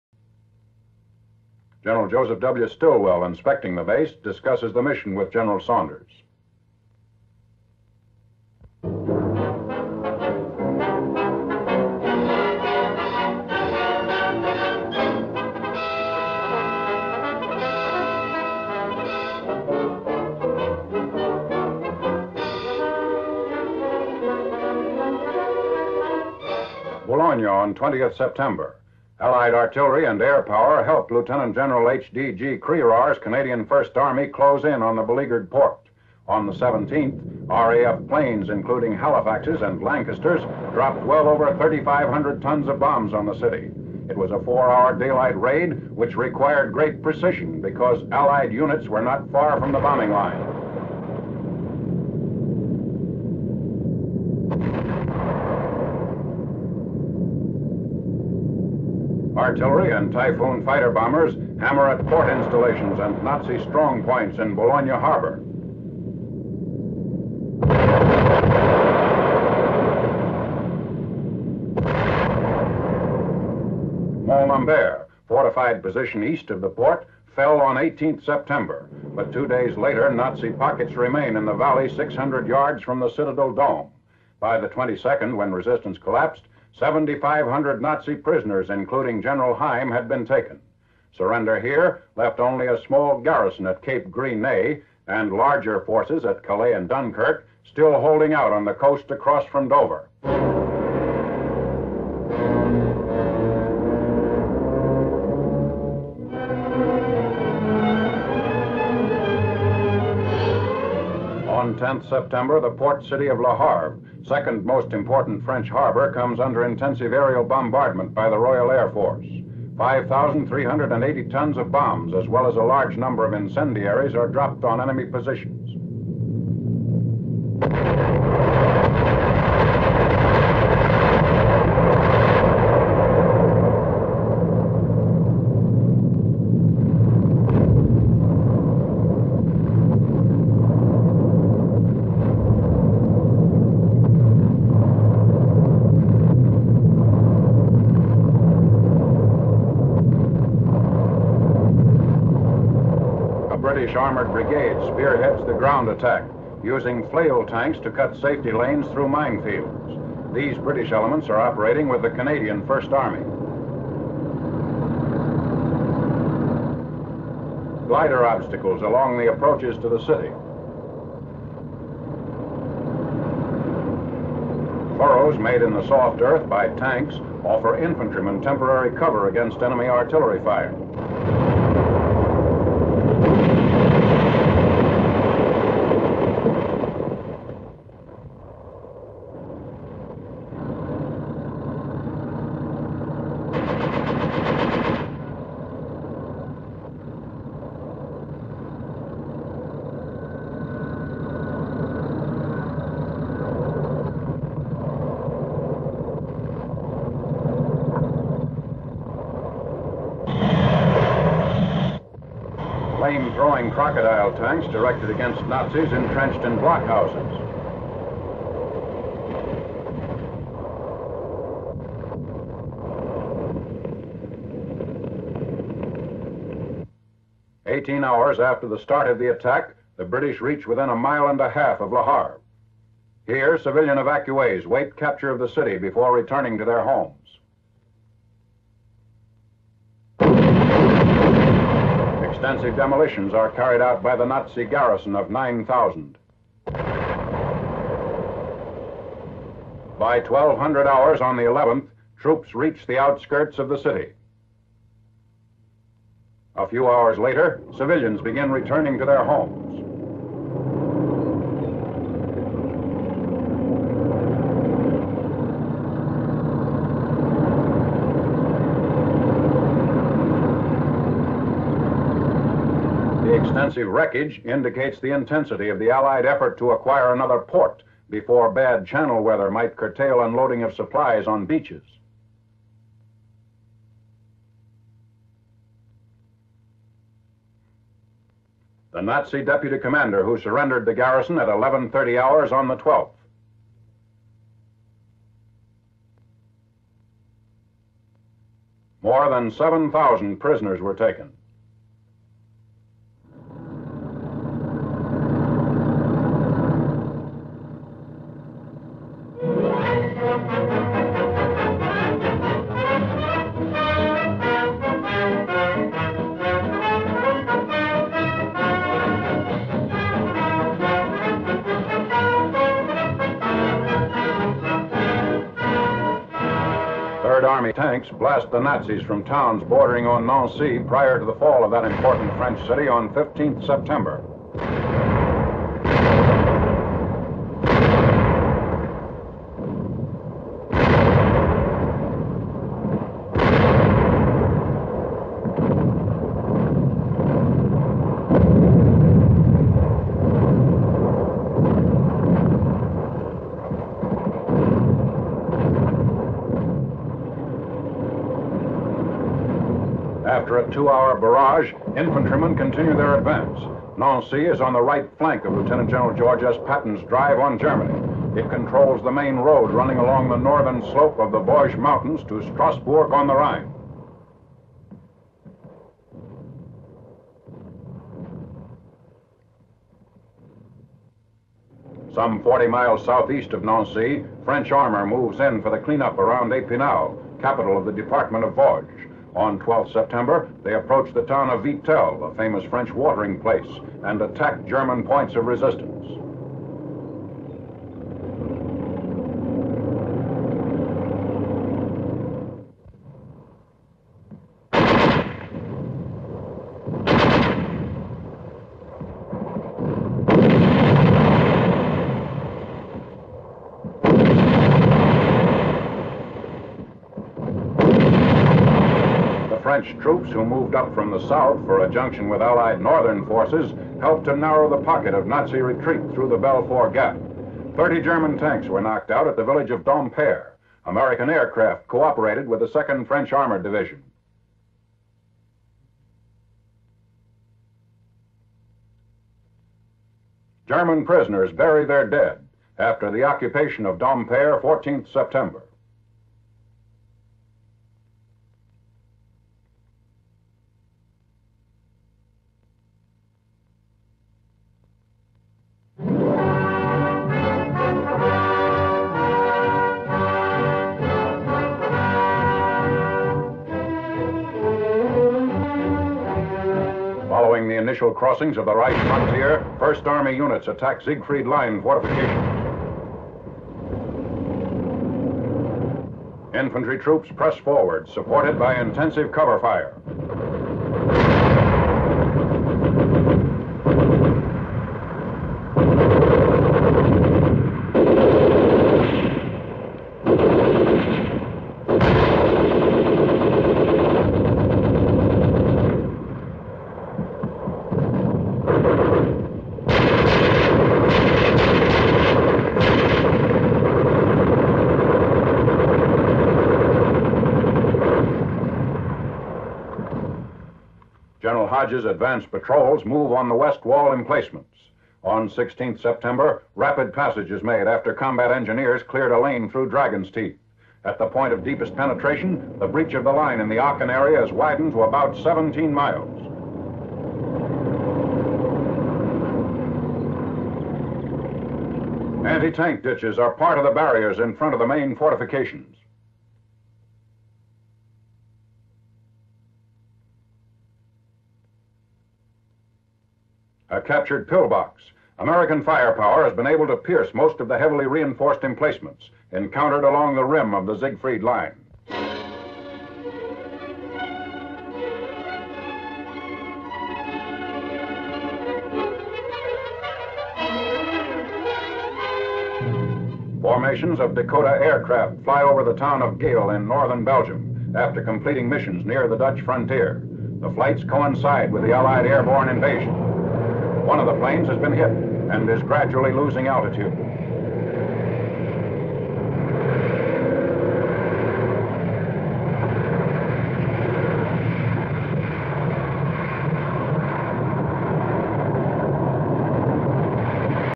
Classified films of World War II Part 8
The United States government produced thousands of films during World War II, many of which were considered "classified." These Film reports covered all theaters of operation and ran the gamut, from important news of battles and invasions, to the introduction of new equipment and supply methods. Originally produced for commanding officers and their immediate staff, we are able to bring you these inside "secret" stories of world War II through the Freedom of Information Act.